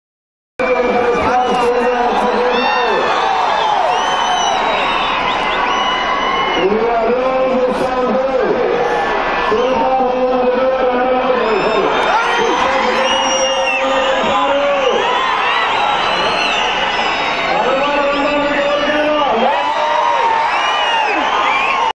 Mr Chandrababu Naidu's Speech in Atmakur (Kurnool)
A 360 pan of the massive crowd gathered at Goud centre for his speech.